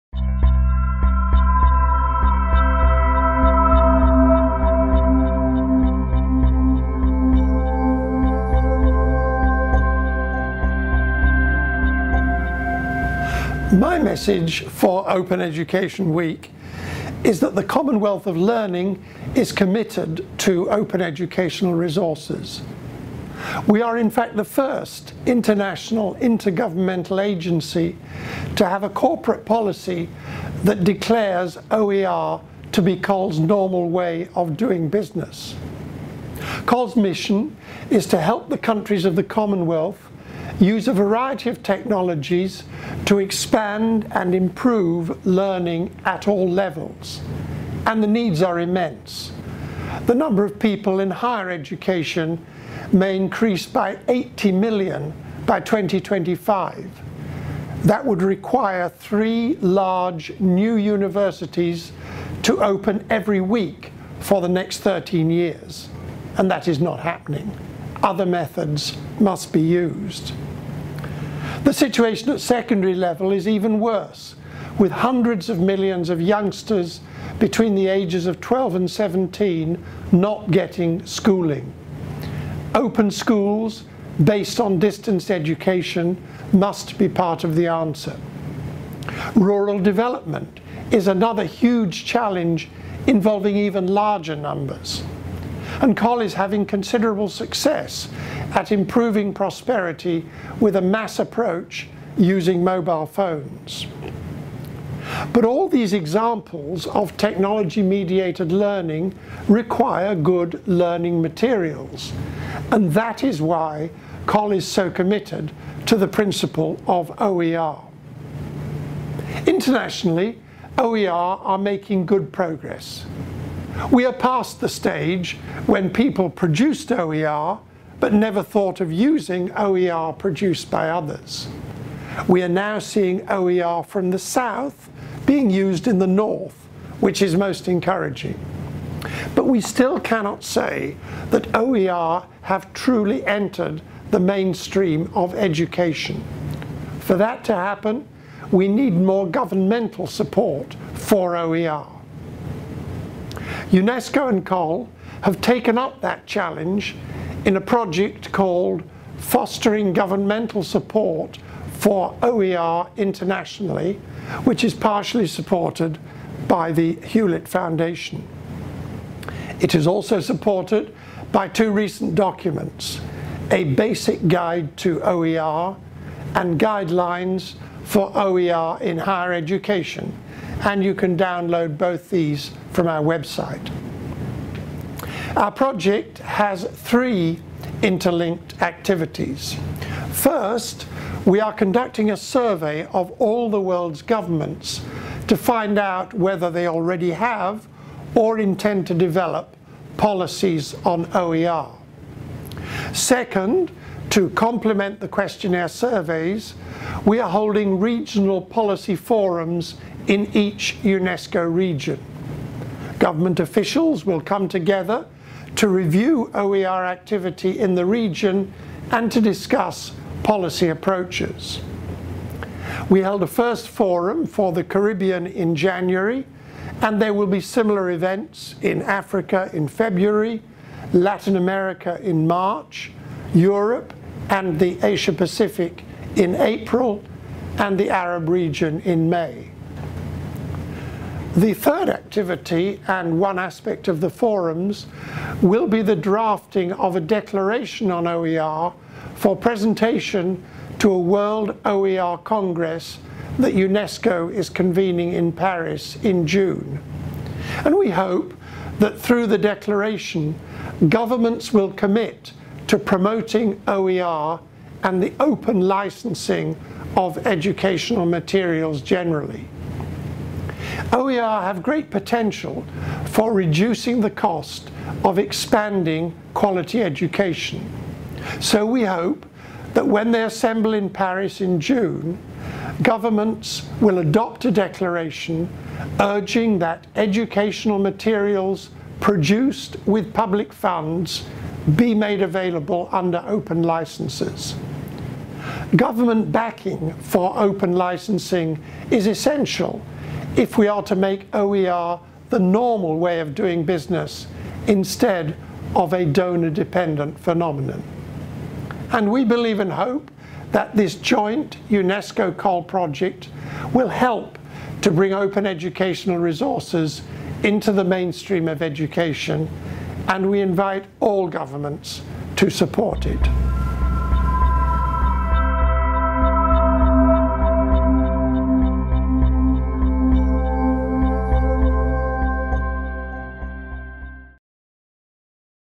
Getting Governments to Back OERs: A joint COL-UNESCO project
Video presentation by Sir John Daniel, President and CEO, Commonwealth of Learning for Open Education Week, 5 - 10 March 2012 (Worldwide).